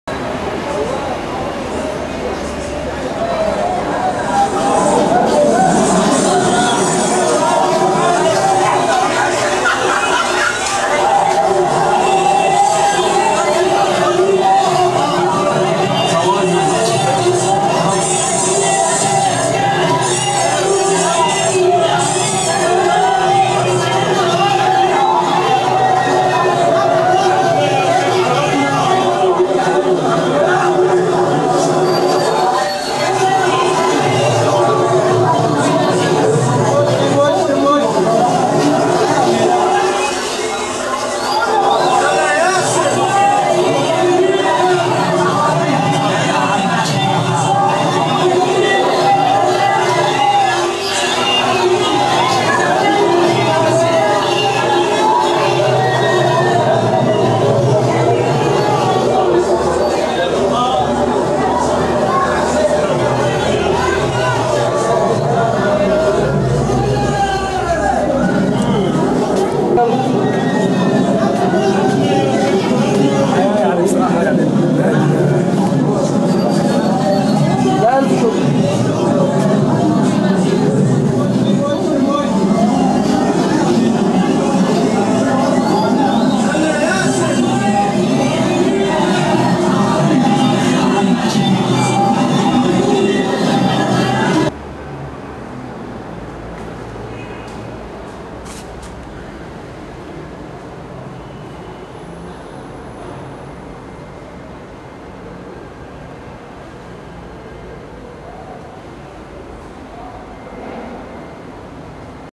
Faces | Video installation by Mohamed Shoukry
Sheets of white cloth suspended in a wooden tunnel, built in the main entrance of Goethe institute Cairo with rear video projection. The tunnel gives a sudden feeling of the street with all its noise, you have to force your way in to the building by removing the cloth and going against the crowd.
The wooden construction built on the entrance door and the staircase - Goethe institute Cairo.
People forcing their way through the tunnel, becoming part of the installation as the video is projected on them as well.